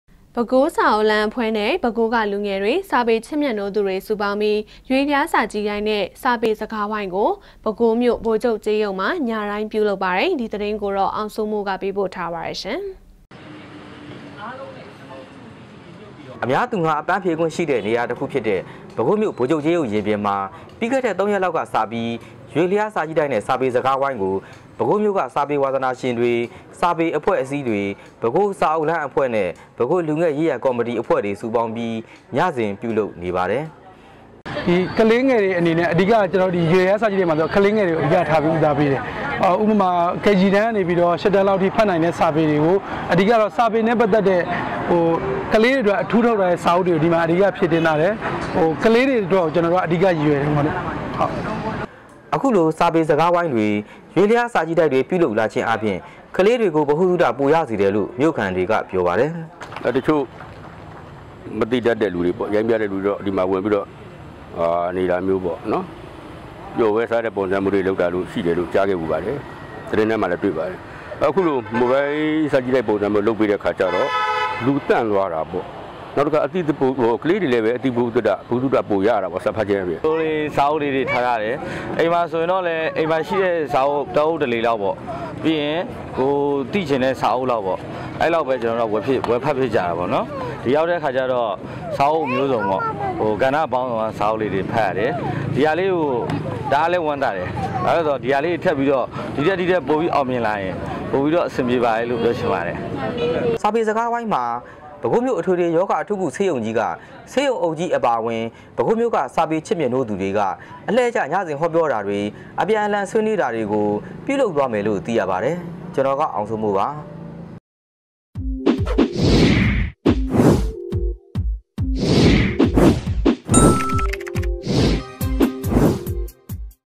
DVB - စာေပခ်စ္သူေတြ စုေပါင္းၿပီး ေ႐ြ႕လ်ားစာၾကည့္တိုက္ နဲ႔ စာေပစကားဝိုင္းကို ပဲခူးၿမိဳ႕မွာျပဳလုပ္
ပဲခူး စာအုပ်လမ်းအဖွဲ့နဲ့ ပဲခူးကလူငယ်တွေ စာပေချစ်မြတ်နိုးသူတွေ စုပေါင်းပြီး ရွေ့လျားစာကြည့်တိုက် နဲ့ စာပေစကားဝိုင်းကို ပဲခူးမြို့ဗိုလ်ချုပ်ကြေးရုပ်မှာ ညတိုင်း ပြုလုပ်ပါတယ်၊ ဒီသတင်းကို အောင်စိုးမိုးက ပေးပို့ထားပါတယ်၊
DVB TV -